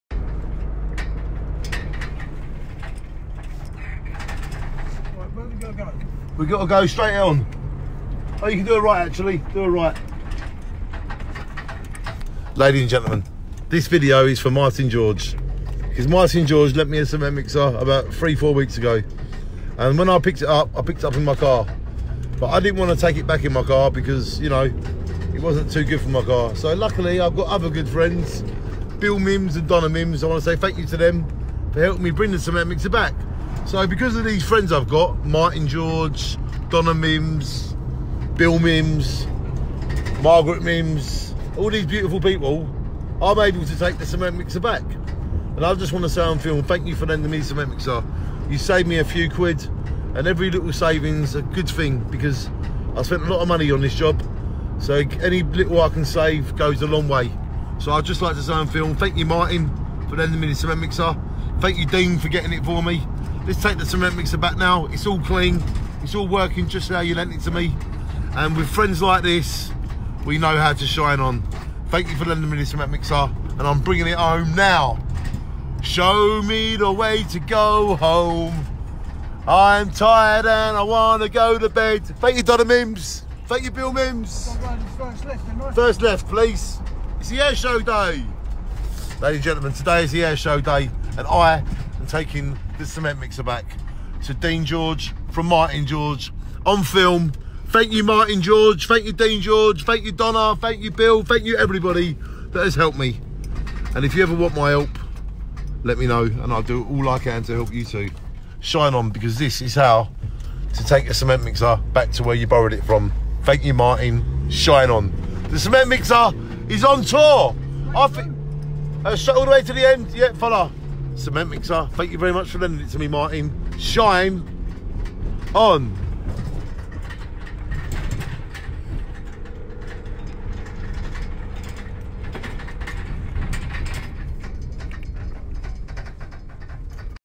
Thankyou Martin George for lending me cement mixer shine on friends real help each other not for mon
Thankyou Martin George for lending me cement mixer shine on friends real help each other not for money for real love